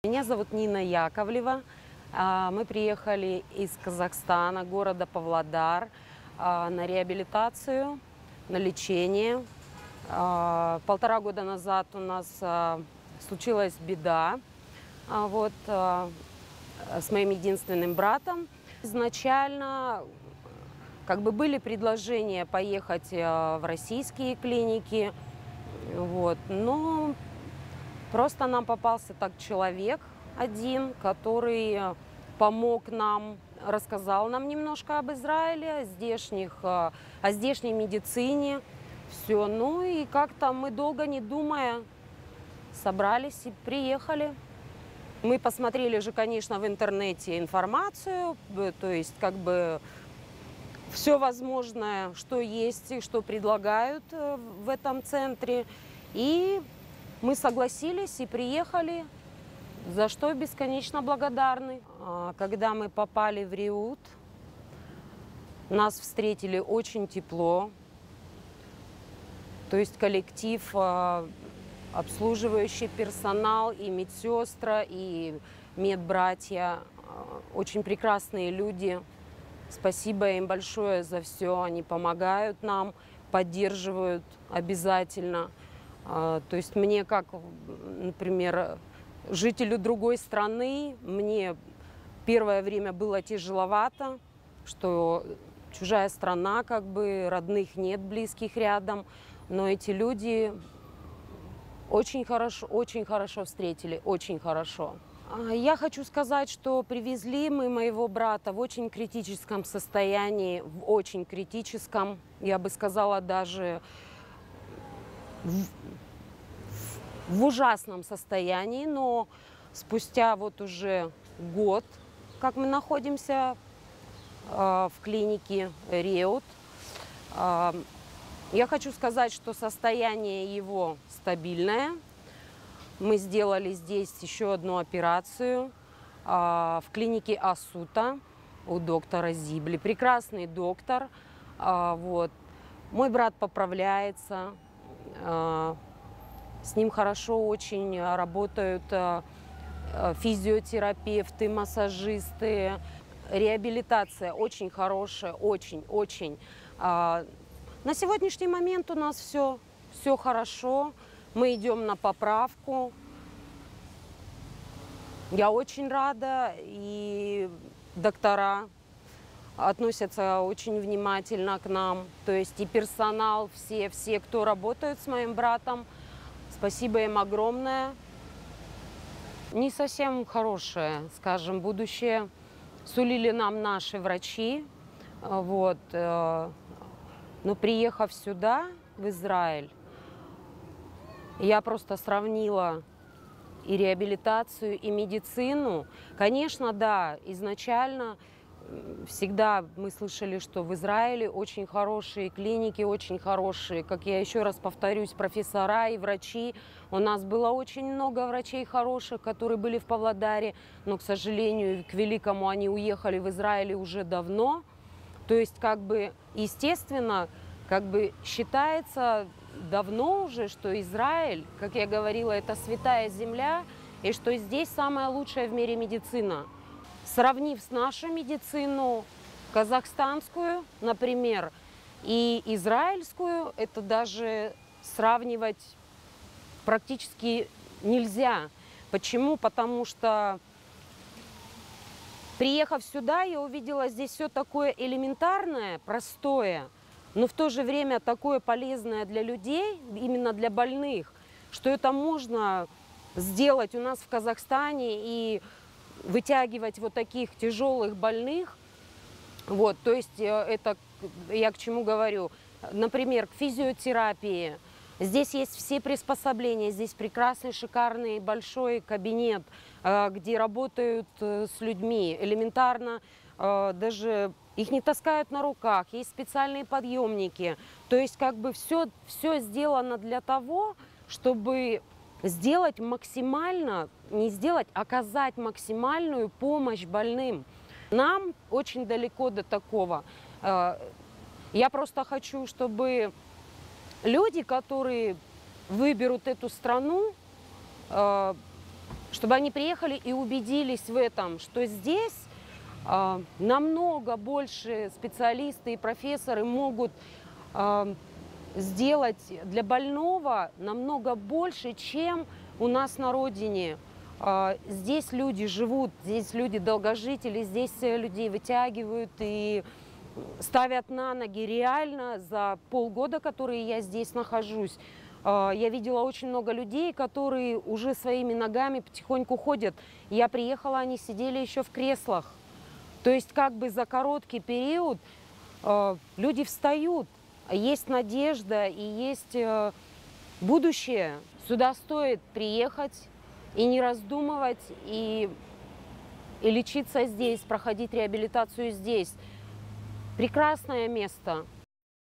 Отзыв семьи пациента о восстановлении в Тель-Авивском реабилитационном центре Реут.
Отзыв о восстановлении пациента из Казахстана, с черепно-мозговой травмой в израильском реабилитационном центре Реут в Тель-Авиве.